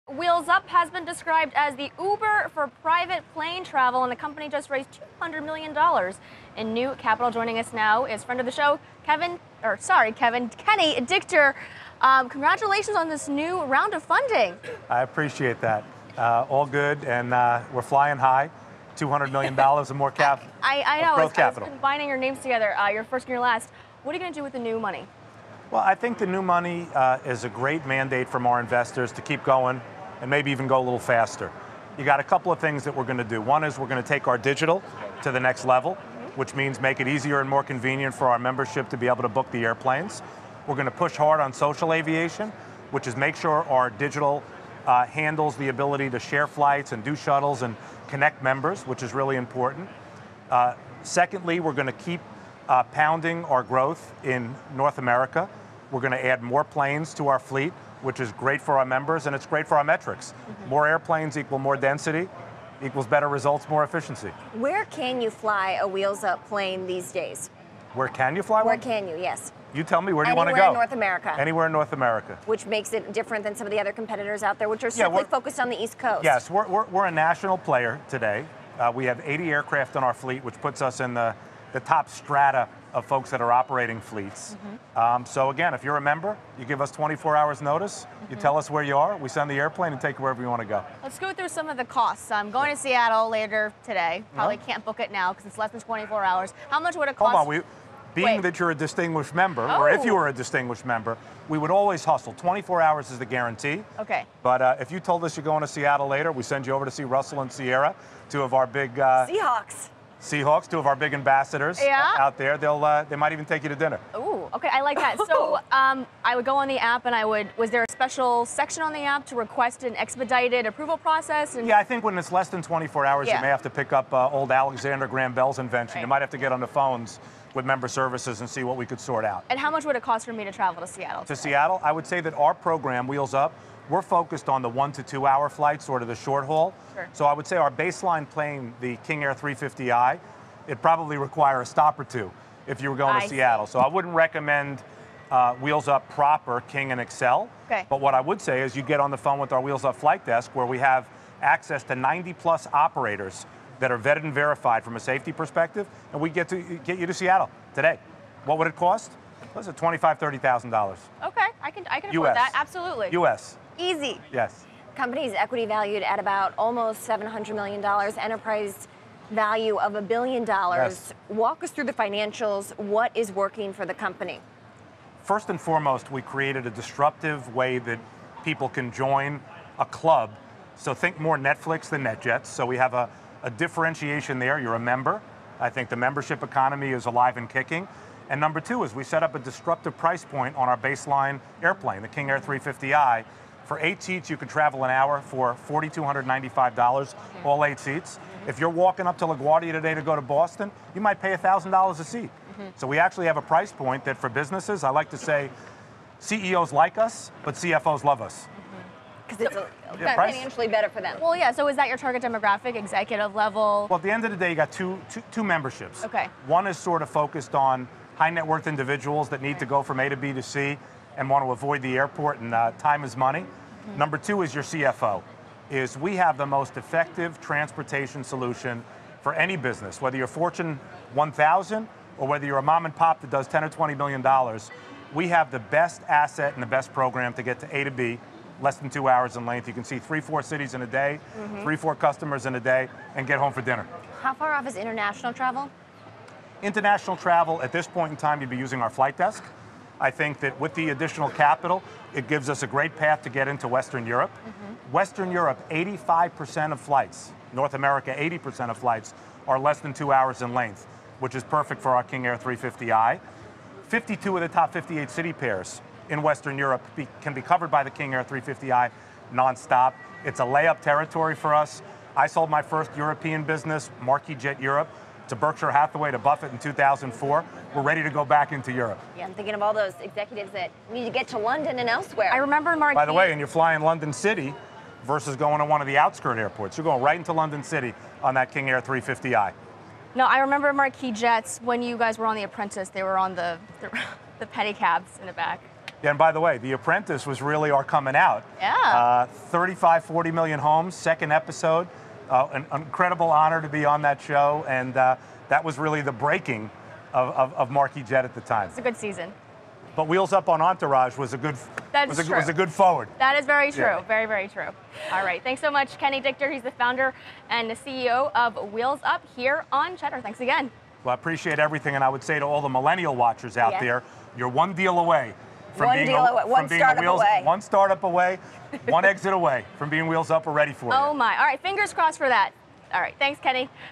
The Uber in the Air: Wheels Up Wheels Up CEO on Cheddar TV 10-16-17
Kenny Dichter, Founder & CEO of Wheels Up, discusses the company's recent $200 million funding round. He says that, with the money, Wheels Up is going to expand social aviation and the ways in which it can connect members. In addition, Wheels Up will keep pounding growth in North America; the company is going to add more planes to the fleet over the next year. He talks about the cost structure and what a flight to Seattle would cost...around $30,000! He says, "CEOs like us, but CFOs love us" because of the cost efficiency.